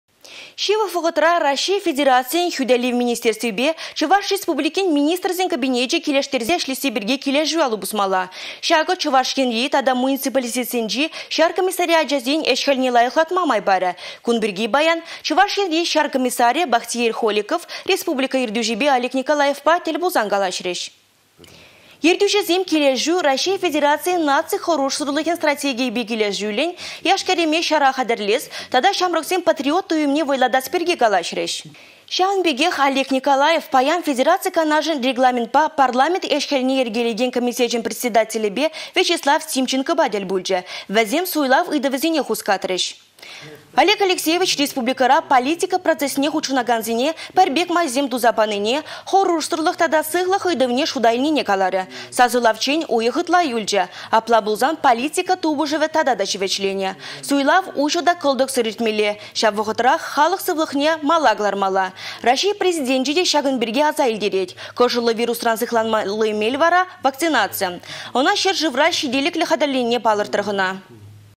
Килĕшӳ пулĕ
Çывăх вăхăтра Раççей Федерацийĕн хӳтĕлев министерствипе Чăваш Республикин Министрсен Кабинечĕ килĕштерсе ĕçлесси пирки килĕшӳ алă пусмалла. Çакă Чăваш Енри  тата муниципалитетсенчи çар комиссариачĕсен ĕç-хĕлне лайăхлатма май парĕ. Кун пирки паян  Чăваш Енри çар комиссарĕ Бахтиер Холиков республика ертӳçипе Олег Николаевпа тĕл пулсан калаçрĕç.